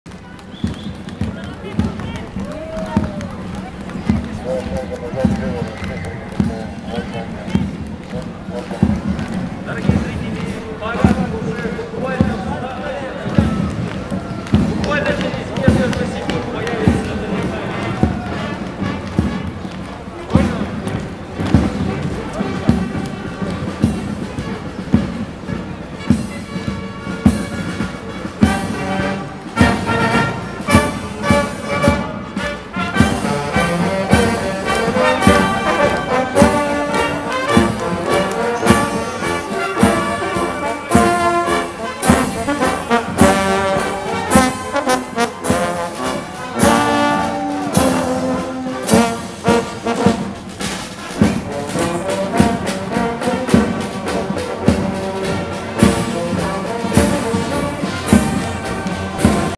Mounted marching band of Tuva - June 5th Fanfari w Azii
one of the rare mounted brass bands in the world... Tuvan national brass band on horses. Taken at June 5th 2014